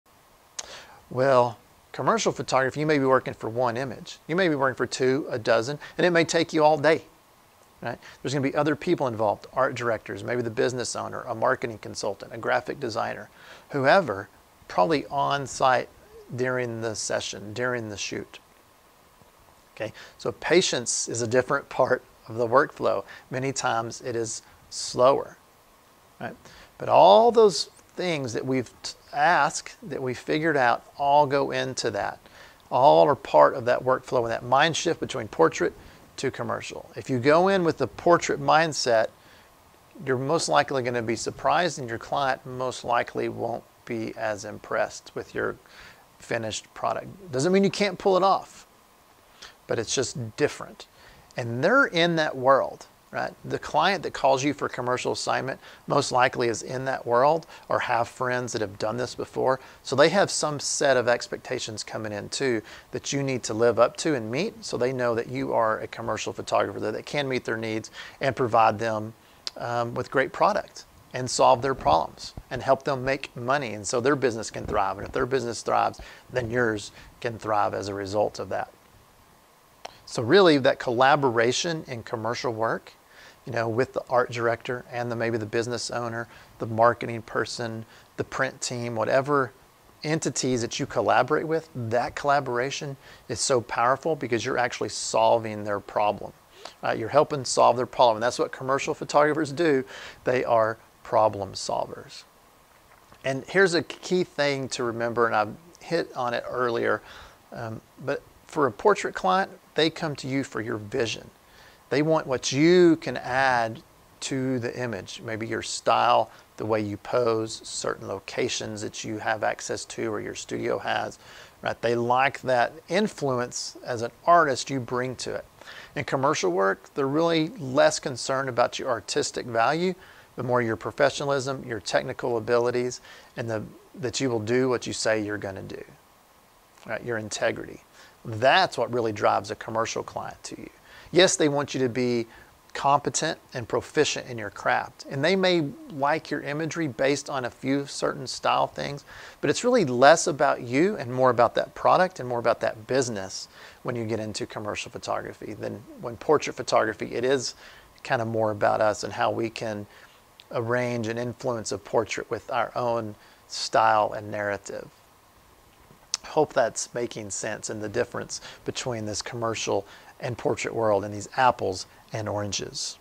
End Use: Cris Duncan
In this section of the Commercial curriculum, @CrisDuncan dives further into commercial assignment logistics.  Specifically targeting tabletop products, this lesson will cover inquiries that you should consider during the planning phases of fulfilling project objectives.

See what's in our Commercial Curriculum here (full content is exclusive to PPA members only)